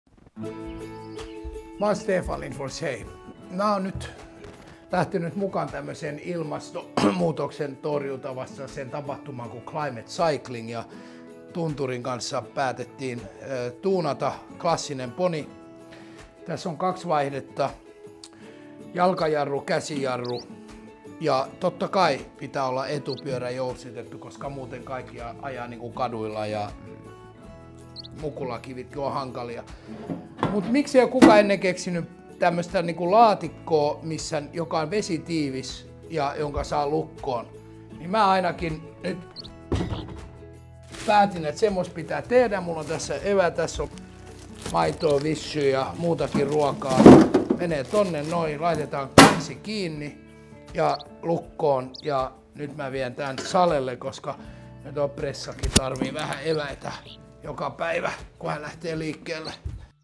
Stefan Lindfors esittelee Tunturille suunnittelemansa Climate Cycling Ponin
No nyt on hieno kaupunkifillari! Muotoilija Stefan Lindfors esittelee videolla Tunturille suunnittelemansa Climate Cycling Ponin.

Useita kymmenillä samanlaisilla CC-pyörällä poljettiin keskiviikkona 11.9. myös kansainvälinen ministeriöiden, yritysten ja yhteisöjen Climate Cycling -tapahtumareitti Helsingissä. Tapahtuman avulla kerätyt varat suunnataan WWF Suomen ilmastotyöhön.

Ensimmäinen pyöristä lahjoitetaan Climate Cycling -tapahtuman viralliselle suojelijalle, presidentti Sauli Niinistölle.

Yksi näistä upeista CC-kaupunkifillareista on nyt arvonnassa! Osallistut arvontaan Tunturin Facebook- tai Instagram-sivuilla Stefanin CC-postauksen yhteydessä.

Kilpailun järjestää Tunturi-Hellberg Oy. Facebook, Instagram tai Youtube eivät ole mukana arvonnassa. Pyörän voittaja julkaistaan perjantaina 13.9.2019 Tunturin Facebookissa ja Instsgramissa.

Tehdään yhdessä ilmastotekoja ja pyöräillään mahdollisimman paljon!